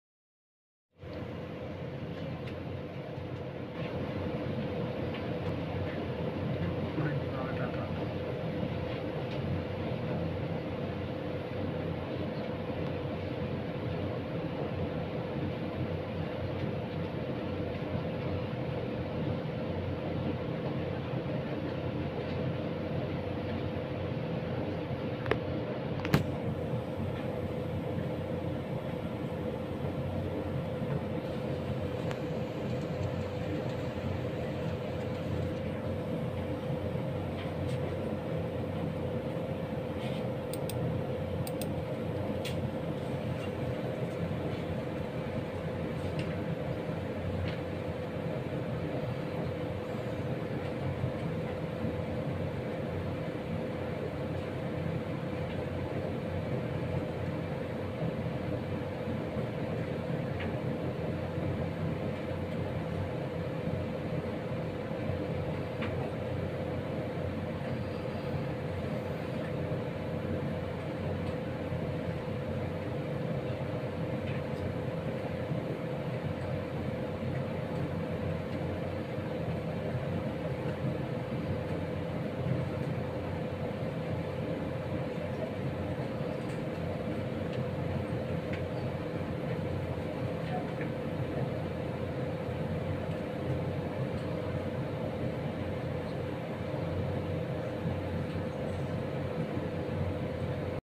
funny Fight of two employees in office between shift time
fight between two employees in working hours